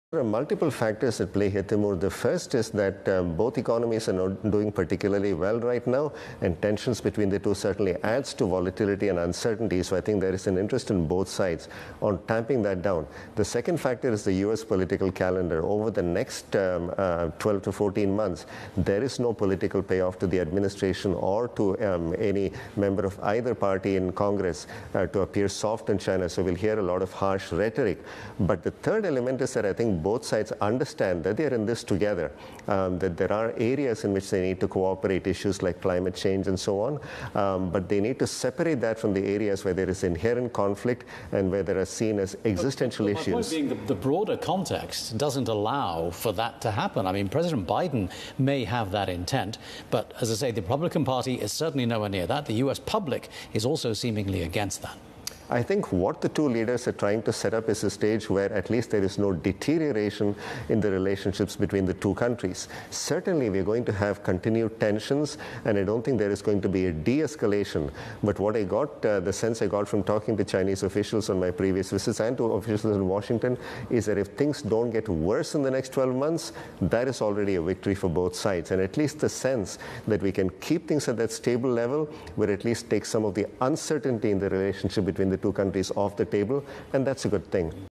Preventing further escalation is a 'victory' for both the US and China: Cornell professor
Cornell University’s Eswar Prasad says the U.S. and China understand there are areas the two countries need to cooperate on, despite existing tensions between.